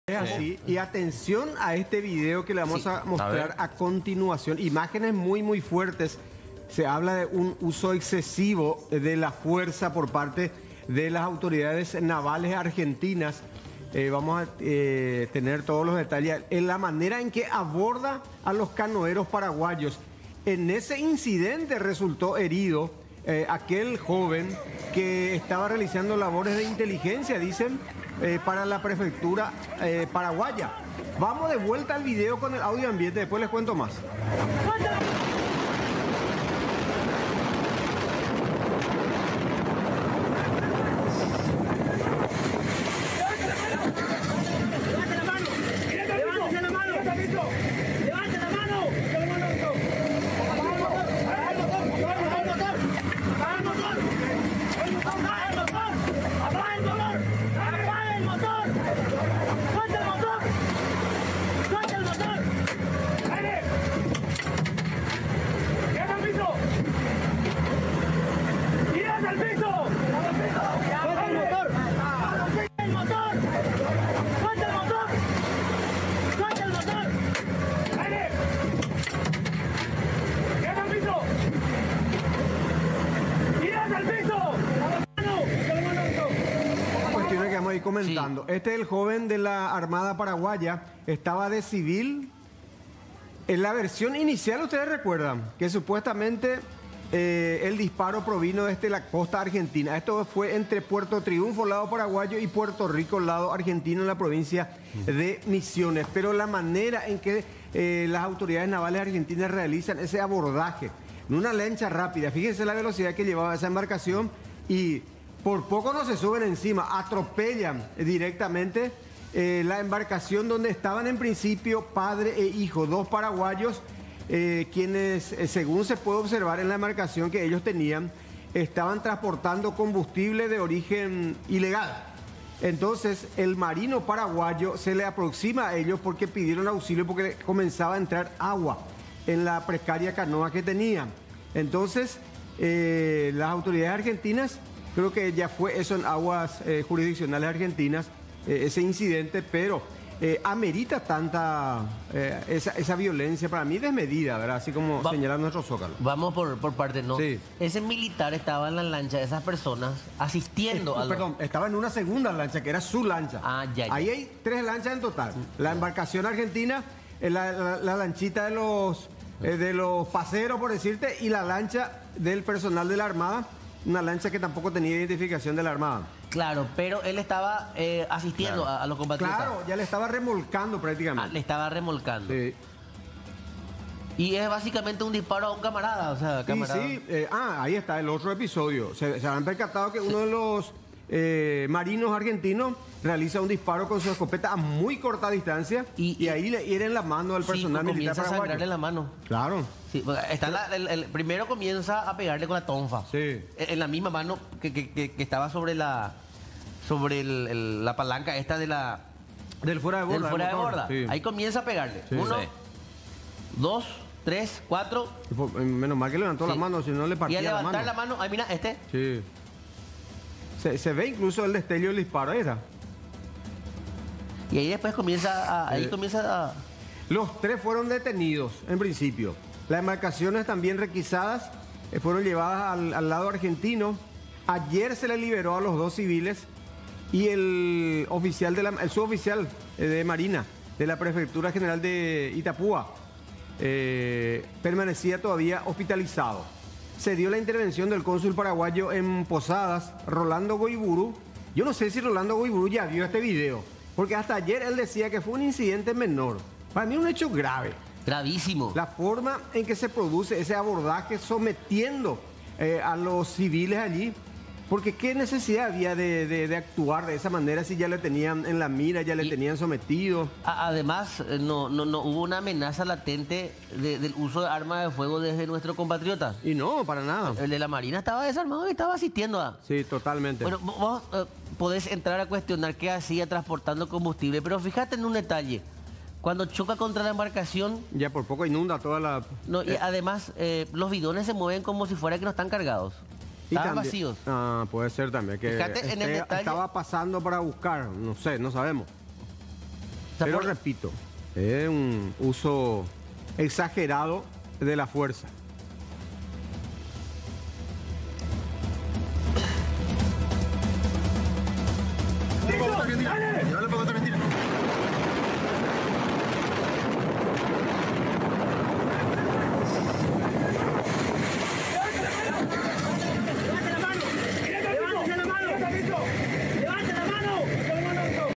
Violencia desmedida en aguas compartidas
El contraalmirante Lucio Benítez comenta sobre lo sucedido el día de ayer, resaltando que los lugareños quieren traer productos que no están permitidos por contrabando